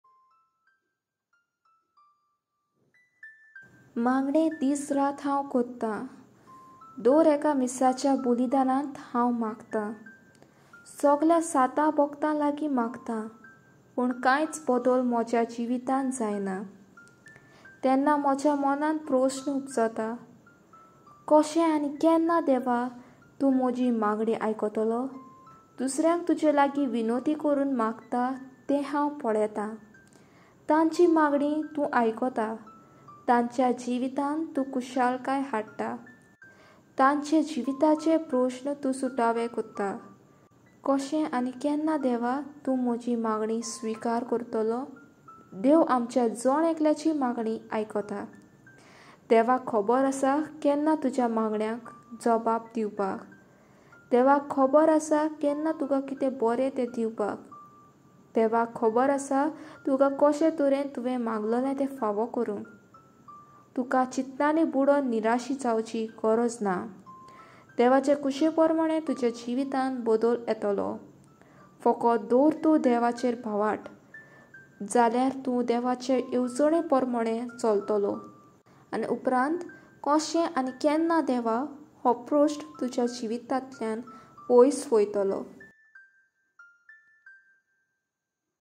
Koxem Ani Kednam, Deva??....Poem by:Shaynie Mascarenhas|Konkani Poetry|Spoken word poetry
KoxemAniKednam,Deva?
poem#SpokenWordPoetry#ShaynieMascarenhas